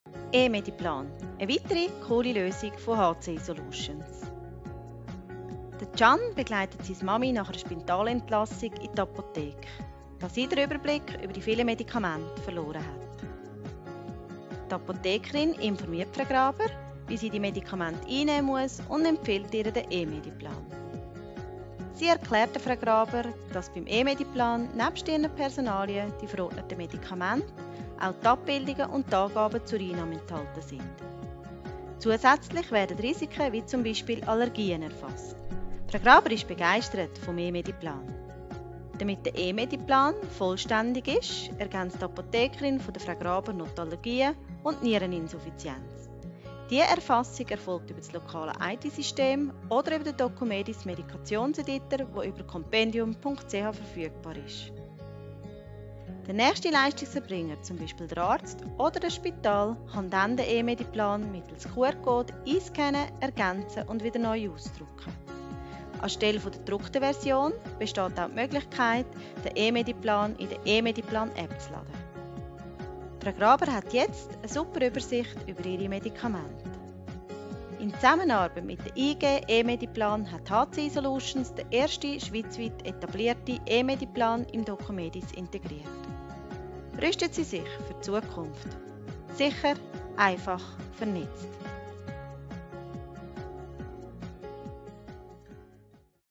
eMediplan - fördert die Compliance
Der eMediplan fasst die gesamte, aktuelle Medikation eines Patienten übersichtlich zusammen. Er eignet sich hervorragend für die Ein- und Austrittsmedikation in Spitälern, Arztpraxen, Heimen und Spitex-Betrieben und fördert die Zusammenarbeit unter den Leistungserbringern.